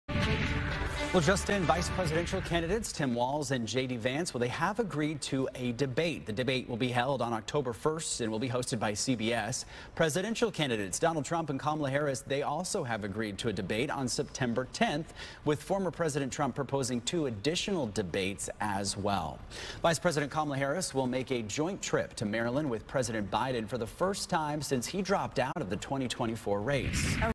Tim Walz, JD Vance agree to October debate
Minnesota Gov. Tim Walz and Ohio Sen. JD Vance have agreed to debate each other on Oct. 1, setting up a matchup of potential vice presidents as early voting in some states gets underway for the general election.